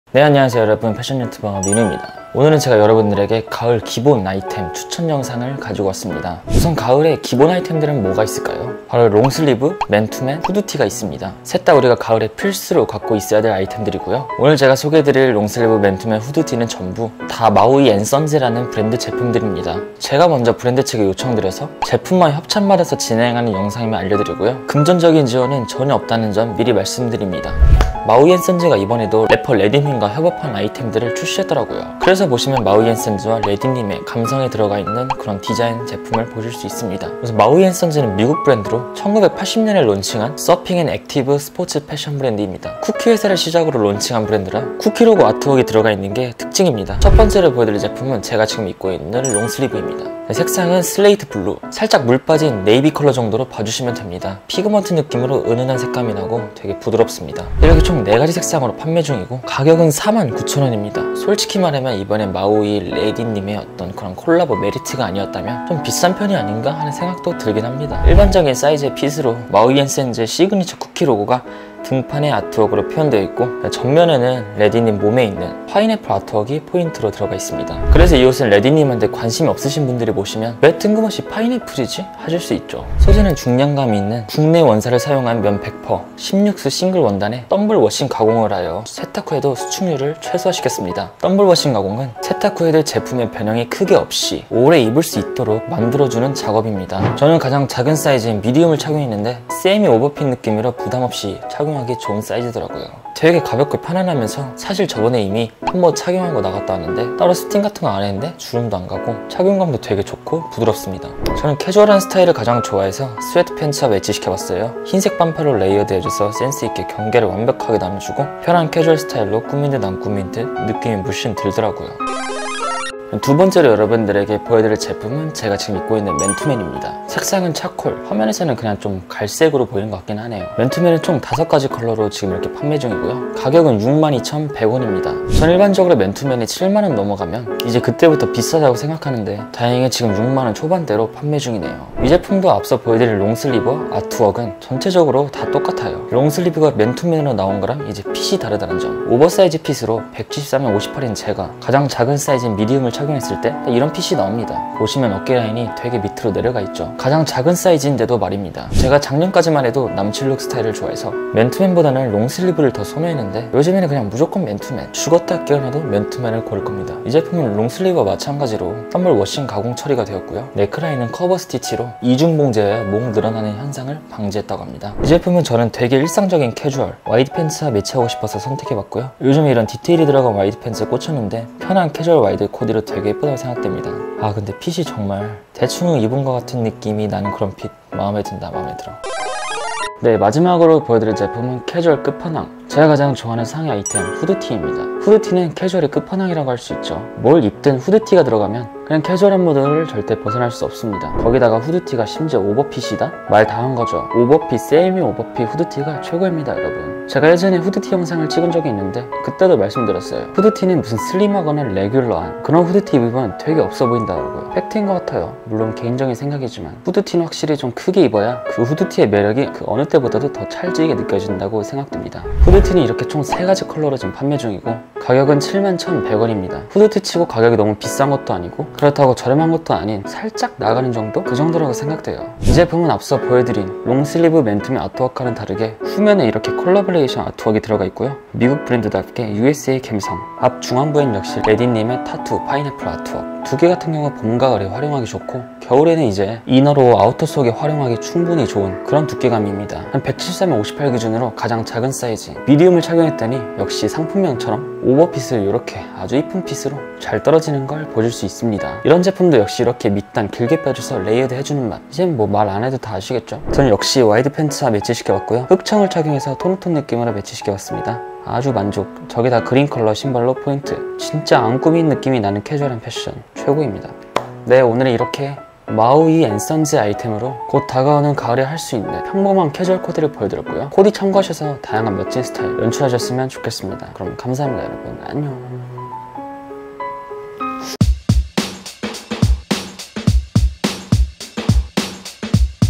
남자 가을 기본 코디 / 가을 캐주얼 패션 / 추천 및 코디
미누 Insta: minuooooooo

안녕하세요 여러분 패션유튜버 미누입니다
오늘은 제가 남자 가을 기본 상의 아이템 추천 및 코디를 준비했습니다
진행하는 영상임을 참고해주시구요
더 궁금하신 점은 댓글 달아주시면 감사하겠습니다!